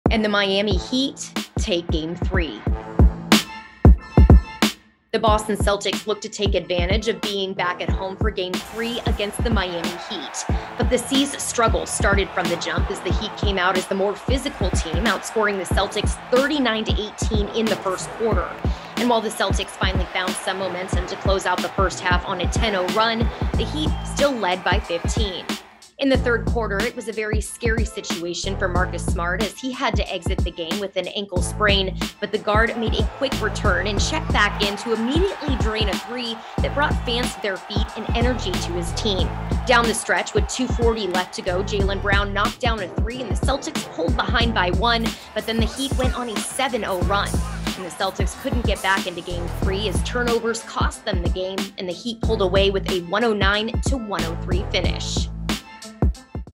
5/21 Putnam Postgame Report: Playing Catchup
After a lackluster start the Celtics were able pull within one point, but the Heat proved to be the more physical team, as they closed out with 109-103 finish.